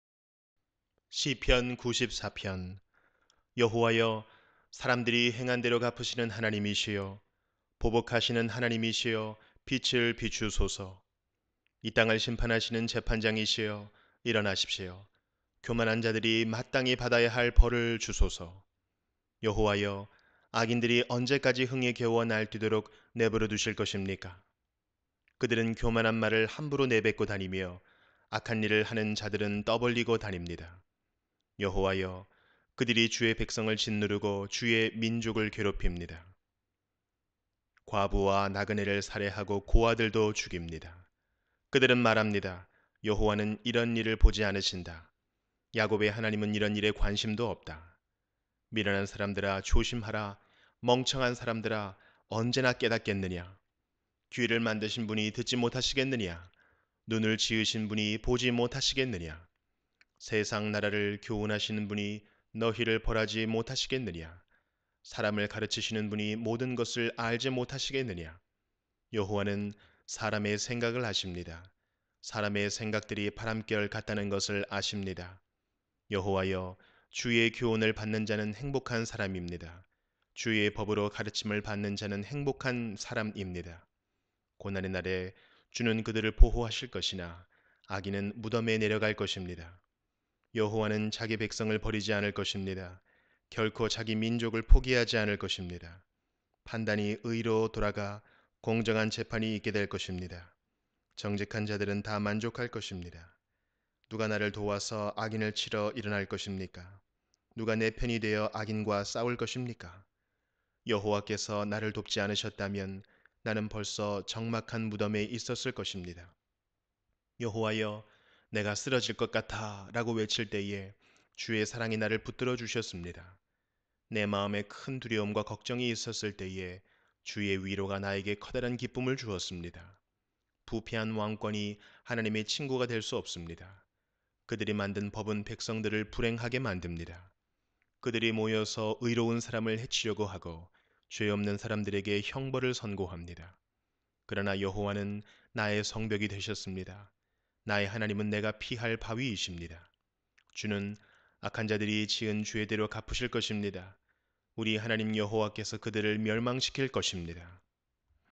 시편 94편 (쉬운성경) Psalm chapter 94
[시]94:1 여호와여, 사람들이 행한 대로 갚으시는 하나님이시여, 보복하시는 하나님이시여, 빛을 비추소서.
[시]94:2 이 땅을 심판하시는 재판장이시여, 일어나십시오. 교만한 자들이 마땅히 받아야 할 벌을 주소서.
[시]94:3 여호와여, 악인들이 언제까지 흥에 겨워 날뛰도록 내버려 두실 것입니까?
[시]94:4 그들은 교만한 말을 함부로 내뱉고 다니며, 악한 일을 하는 자들은 떠벌리고 다닙니다.
[시]94:5 여호와여, 그들이 주의 백성을 짓누르고, 주의 민족을 괴롭힙니다.
[시]94:6 과부와 나그네를 살해하고, 고아들도 죽입니다.
[시]94:7 그들은 말합니다. "여호와는 이런 일을 보지 않으신다. 야곱의 하나님은 이런 일에 관심도 없다."
[시]94:8 미련한 사람들아, 조심하라. 멍청한 사람들아, 언제나 깨닫겠느냐?
[시]94:9 귀를 만드신 분이 듣지 못하시겠느냐? 눈을 지으신 분이 보지 못하시겠느냐?
[시]94:10 세상 나라를 교훈하시는 분이 너희를 벌하지 못하시겠느냐? 사람을 가르치시는 분이 모든 것을 알지 못하시겠느냐?
[시]94:11 여호와는 사람의 생각을 아십니다. 사람의 생각들이 바람결 같다는 것을 아십니다.
[시]94:12 여호와여, 주의 교훈을 받는 자는 행복한 사람입니다. 주의 법으로 가르침을 받는 자는 행복한 사람입니다.
[시]94:13 고난의 날에 주는 그들을 보호하실 것이나 악인은 무덤에 내려갈 것입니다.
[시]94:14 여호와는 자기 백성을 버리지 않을 것입니다. 결코 자기 민족을 포기하지 않을 것입니다.
[시]94:15 판단이 의로 돌아가 공정한 재판이 있게 될 것입니다. 정직한 자들은 다 만족할 것입니다.
[시]94:16 누가 나를 도와서 악인을 치러 일어날 것입니까? 누가 내 편이 되어 악인과 싸울 것입니까?
[시]94:17 여호와께서 나를 돕지 않으셨다면 나는 벌써 적막한 무덤에 있었을 것입니다.
[시]94:18 여호와여, 내가 "쓰러질 것 같아!"라고 외칠 때에 주의 사랑이 나를 붙들어 주셨습니다.
[시]94:19 내 마음에 큰 두려움과 걱정이 있었을 때에 주의 위로가 나에게 커다란 기쁨을 주었습니다.
[시]94:20 부패한 왕권이 하나님의 친구가 될 수 없습니다. 그들이 만든 법은 백성들을 불행하게 만듭니다.
[시]94:21 그들이 모여서 의로운 사람을 해치려고 하고, 죄 없는 사람들에게 형벌을 선고합니다.
[시]94:22 그러나 여호와는 나의 성벽이 되셨습니다. 나의 하나님은 내가 피할 바위이십니다.
[시]94:23 주는 악한 자들이 지은 죄대로 갚으실 것입니다. 우리 하나님 여호와께서 그들을 멸망시킬 것입니다.